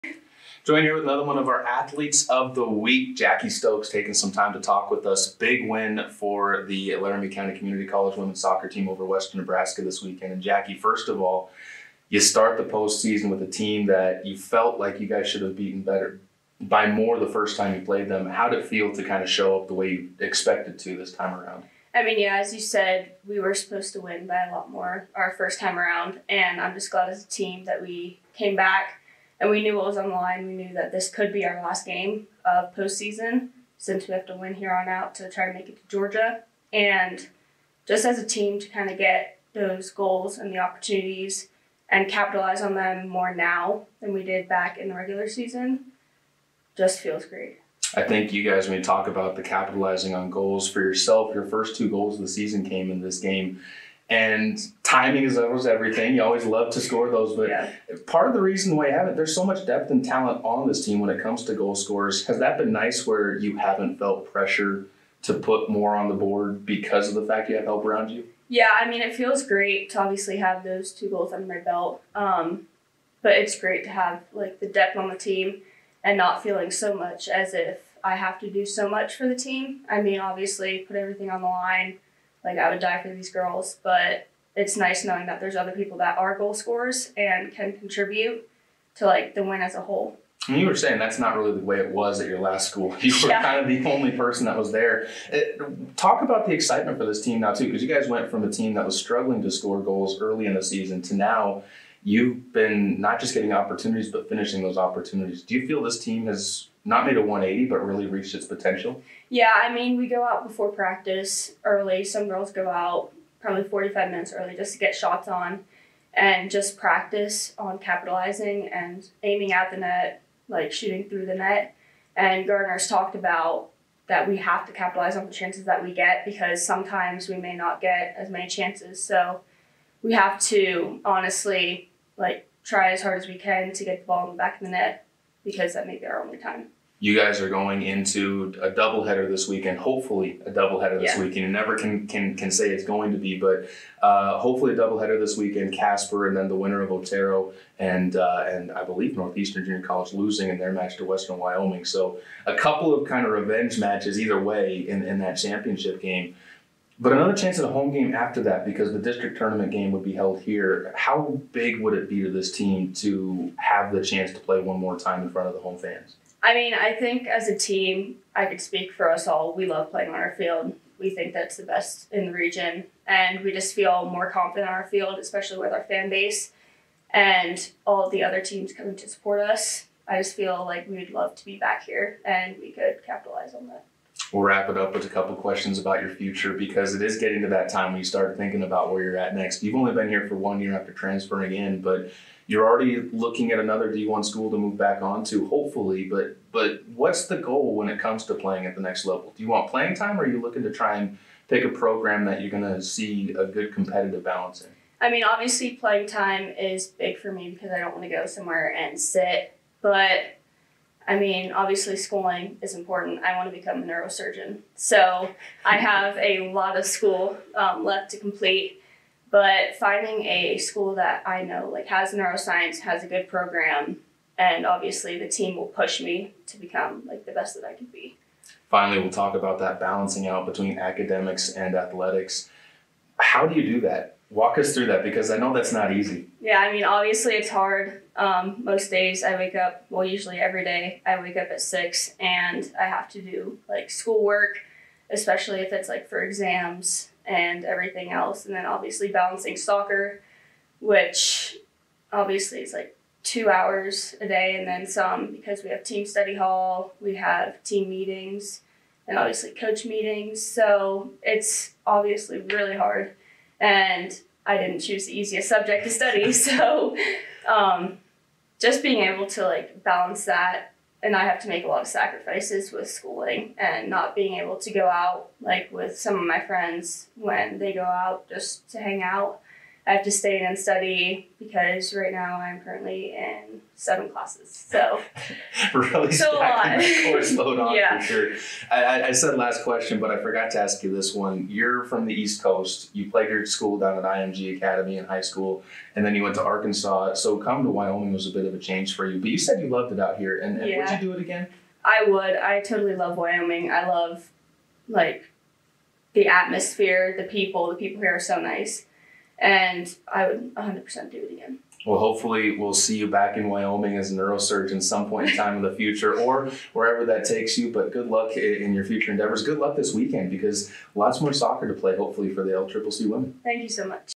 Jacqueline Stokes Oct 25, 2022
We had a chance to talk with our Athlete of the Week from the LCCC Women's Soccer team.
Jacqueline Stokes had a pair of goals in a big win for the Golden Eagles on Saturday to move them to the Region IX Semifinals on Friday night in Pueblo!
Congratulations!